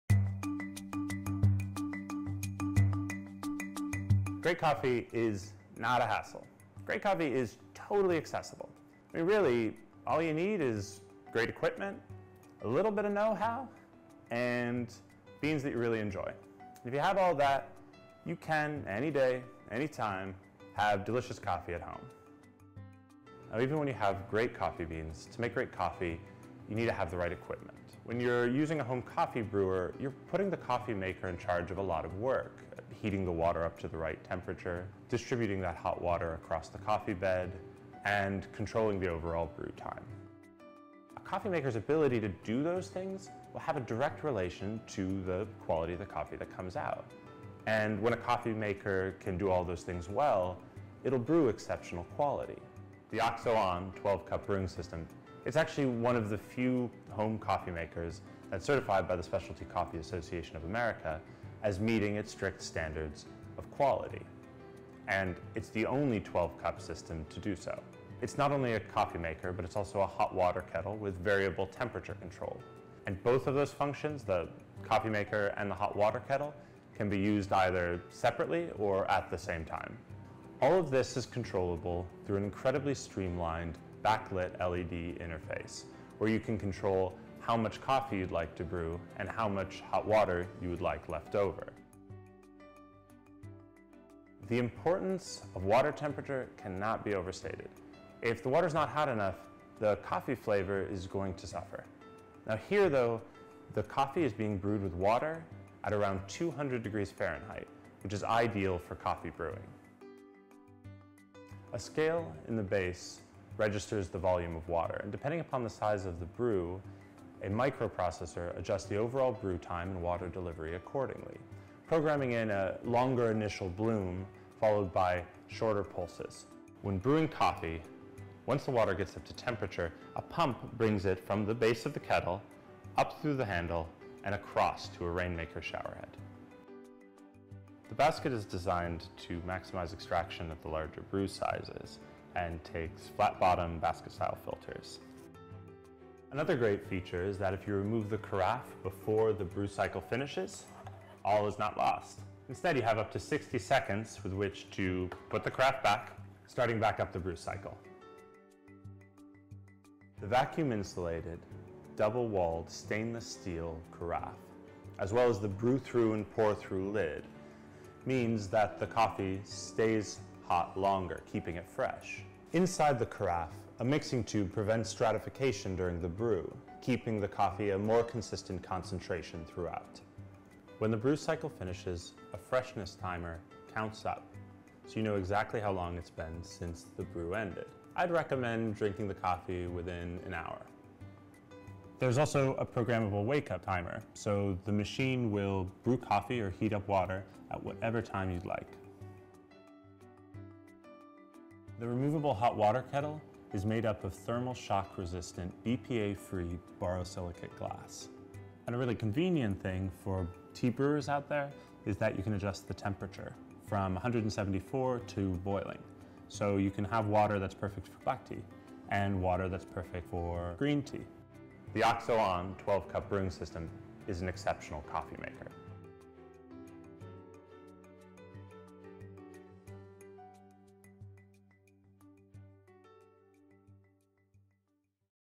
OXO 12-Cup Coffee Brewing System
Brewing delicious coffee is part chemistry, part art. The perfect cup demands a sequence of ideal conditions, and frankly, most home machines aren’t up to the task. Thankfully, the OXO Coffee Brewing System is.

Featuring an intelligent microprocessor, an internal scale and precise temperature control, OXO’s new brewing system delivers the precision of hand-crafted coffee with the ease of ordering at your favorite coffee shop. Simply choose the number of cups, add ground coffee and let the machine to the rest.